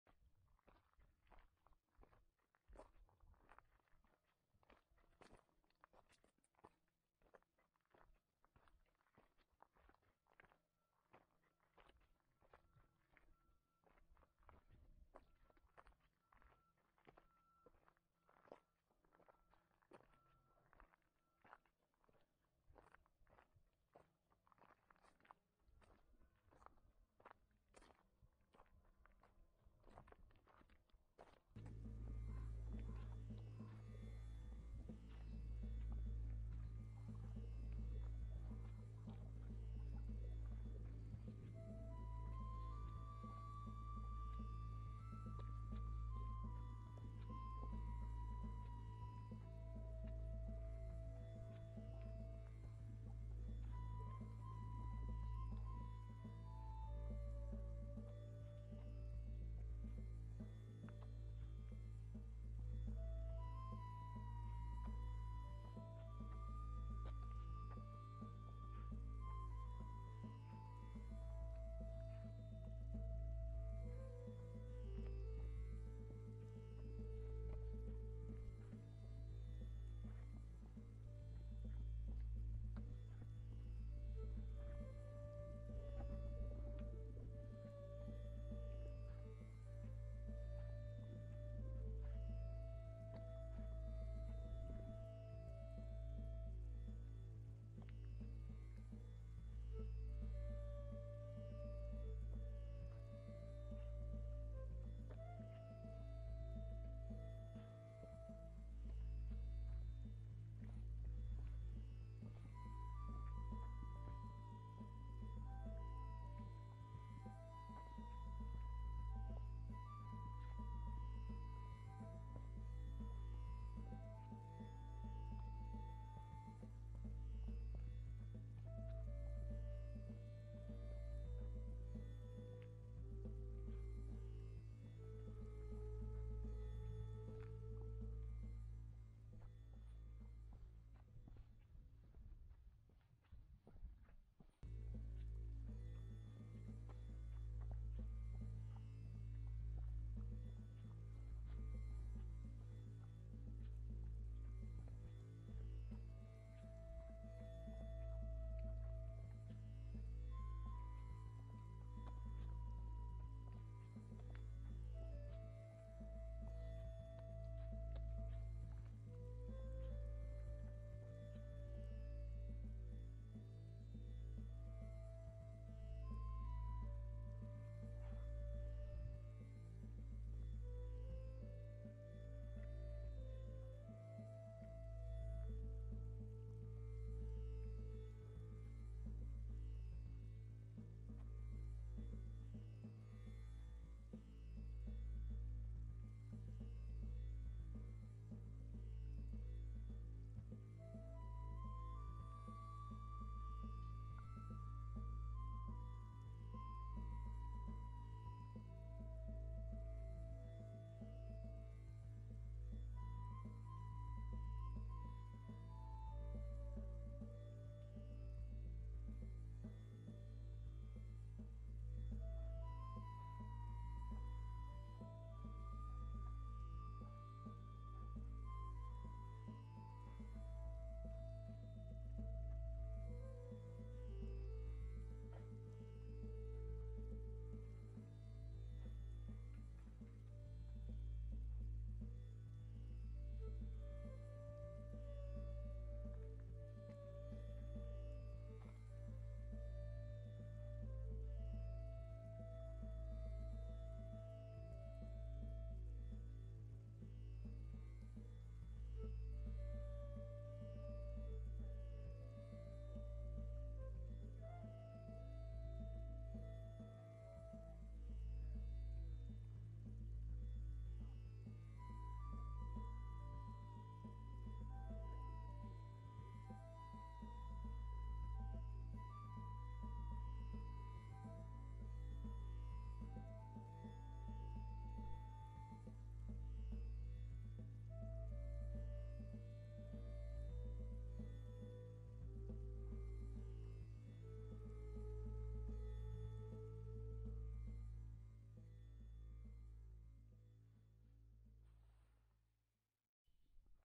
Lake, Saguaro Cactus, Cliff Dwelling Hike (Tonto National Monument, Arizona) [4k]
Lovely hike to the lower cliff dwelling built by Salado people about 1300 CE opens beautiful views of the Roosevelt Lake, giant saguaro cactus and teddy bear cholla. The lake was formed by Theodore Roosevelt Dam on the Salt River in Arizona.